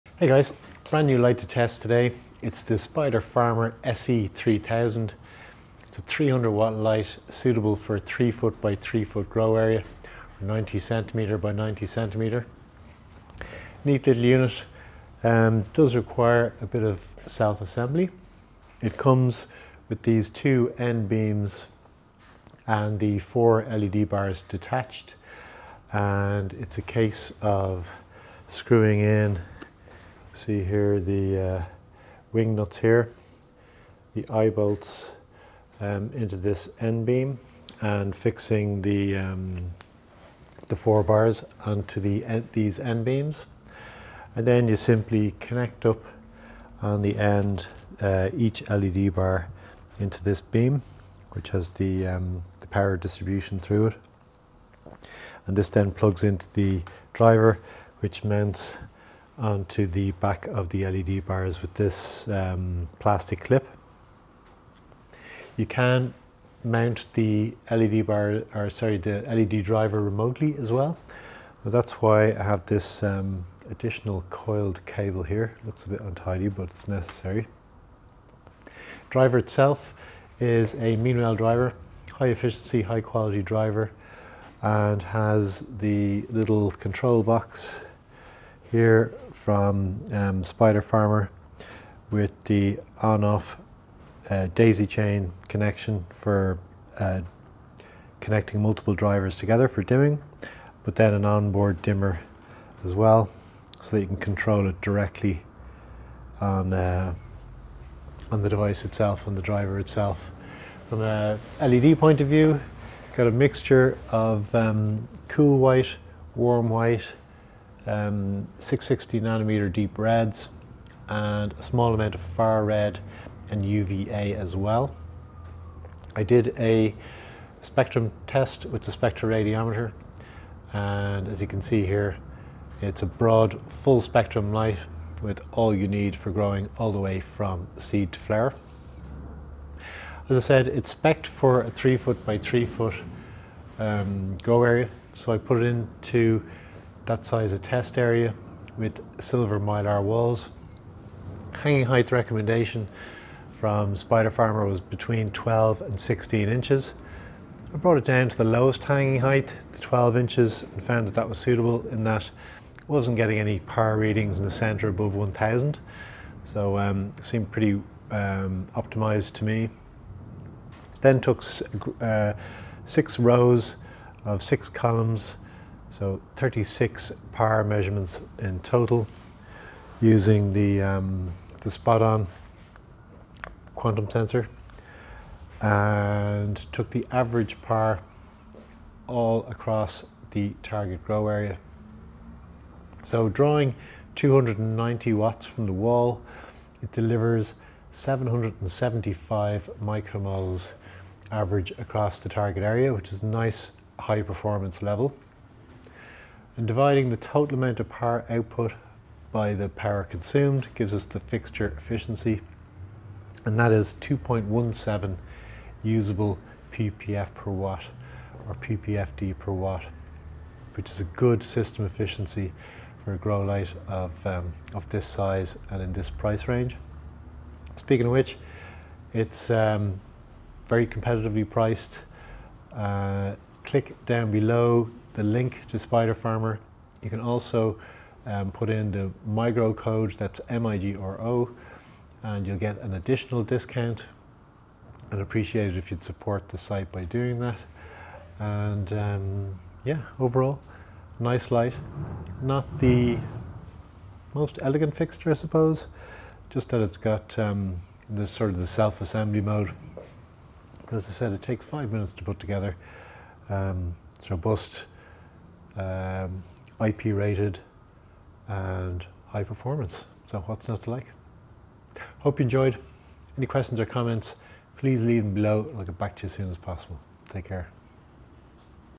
Spider Farmer SE3000 3x3 LED grow light with Samsung LM301B PAR test and review
I review the Spider Farmer SE3000 LED grow light for a 3x3 0r 90cm x 90cm grow space.
The light requires a little assembly which is very easy and quick.
The Meanwell LED driver can be mounted on the back of the bars or remotely, whichever suits your setup. The driver has on board dimming as well as ports for daisy chain dimming control.
The white LEDs are Samsung LM301B with protective coating and there are also added UVA and Far red diodes.
The light was tested in a 3x3 area at 12" or 30cm hanging height. The average PAR was 775 µmols which is a good level and the system efficiency calculated at 2.17 µmols/watt or usable PPF/watt.